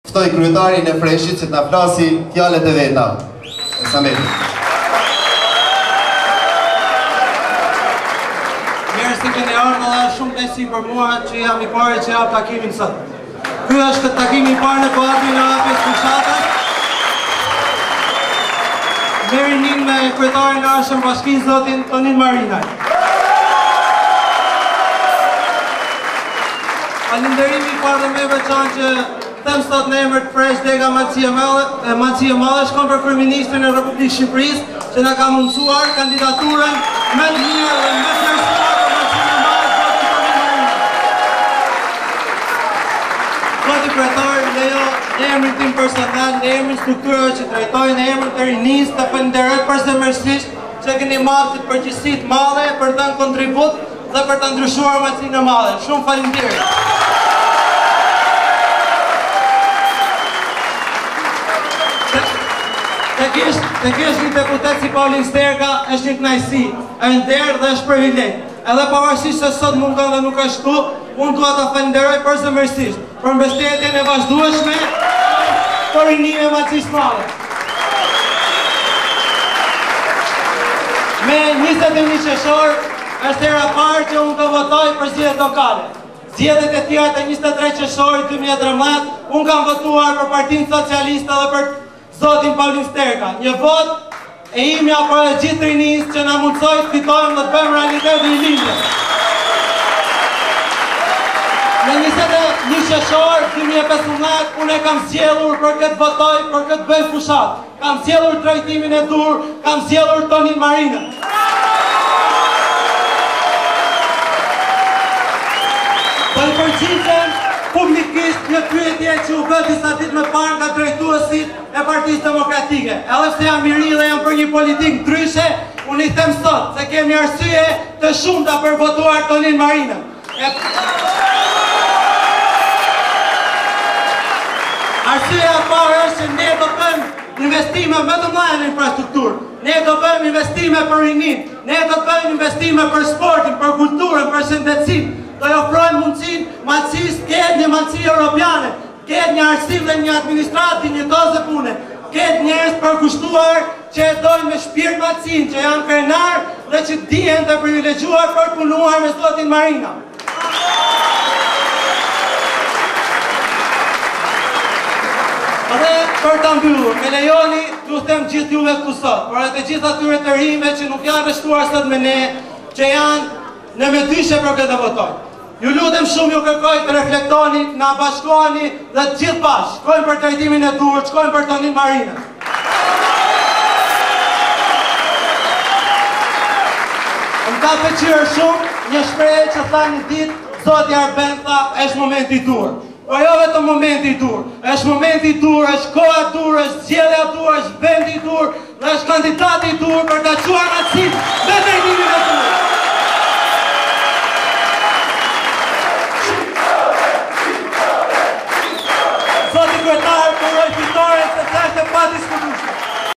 Elsamed Muka Fjalimi ne Takimin Rinor me Tonin Marinaj
+Tonin Marinaj takim me rinine e Malesise se Madhe